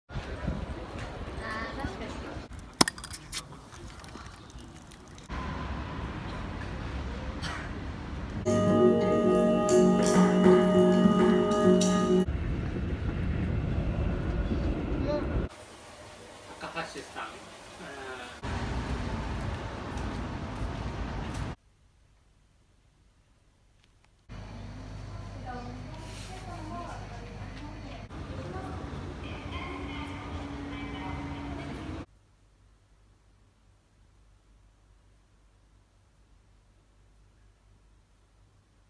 TOKYO LIVING: Spring Diary
My Tokyo Spring 2019 in less than a minute.
一分以内私の東京2019の春です。

Meguro Cherry Blossoms
Ramune
Atago Shrine, Roppongi
Mori Art Museum
Tokyo Metro Train
Japanese Nabe (Hot pot)
Shinjuku
Instant Yakisoba
Mame Shiba Inu Cafe, Akihabara
Mori Art Tower
Instant vermicelli soup